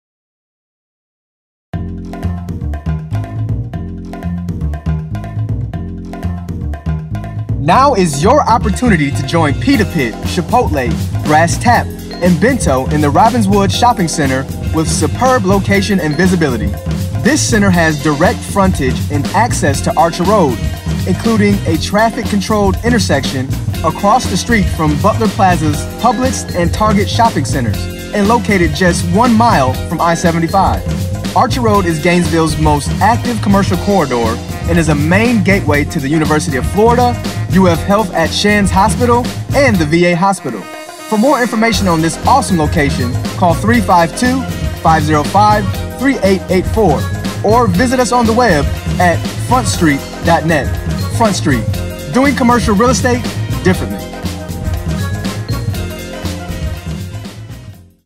Robbinswood - Archer Road Retail Space For Lease in Gainesville, FL
Join Pita Pit, Chipotle, Brass Tap, and Bento in Robbinswood Shopping Center with superb location and visibility.